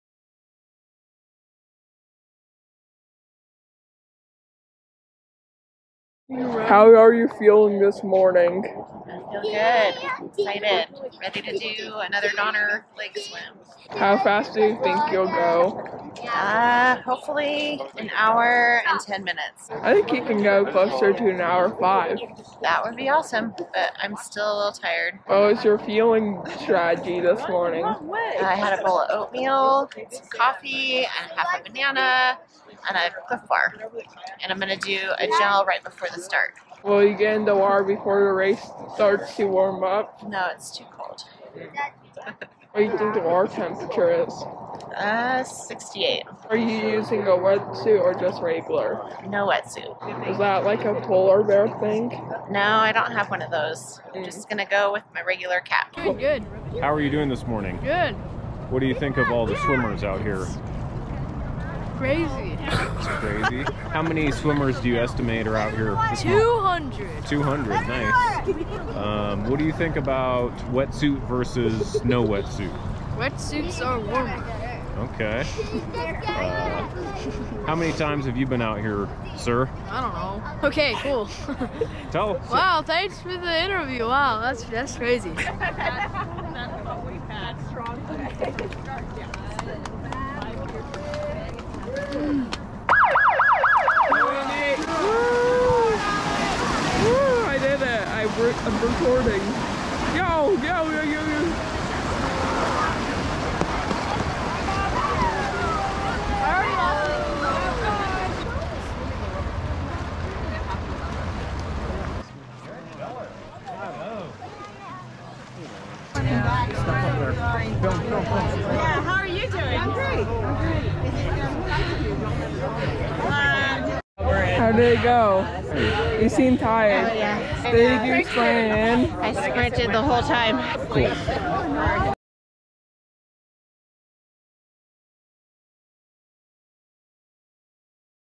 Donner Lake Open Water Swim 2022
A quick re-cap of the Donner Lake Open Water Swim.

A 2.7 mile swim down the length of Donner Lake near Truckee California.

Special thanks to @alexdoesexist for making this video.